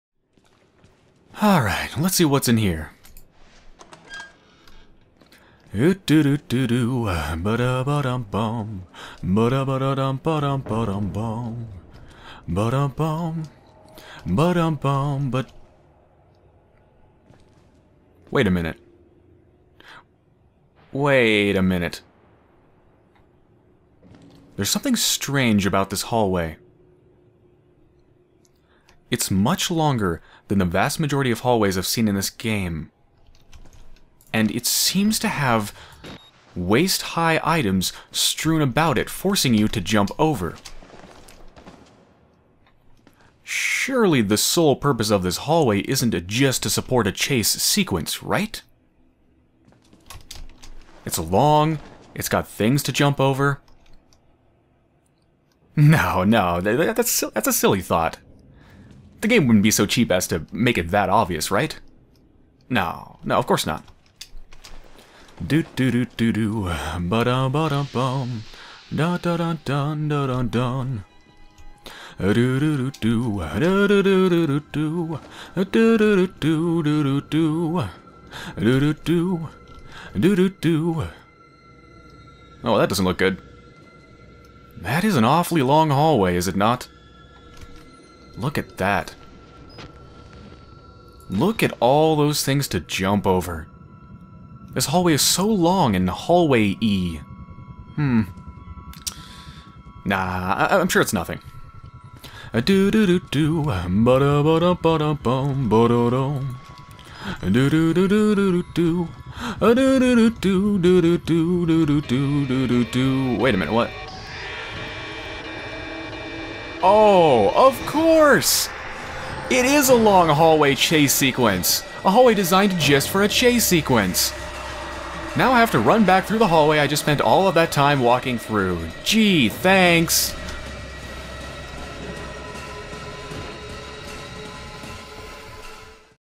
The Suspicious Hallway
There's NO WAY this hallway is designed with the sole purpose of being used for a chase sequence. It's totally not obvious at all. This is totally not an example of bad design. This hallway doesn't at all scream in your face "I WAS MADE FOR ONE PURPOSE. I AM A MOCKERY OF BELIEVABILITY. I SHOULD NOT EXIST. KILL MEEEEE"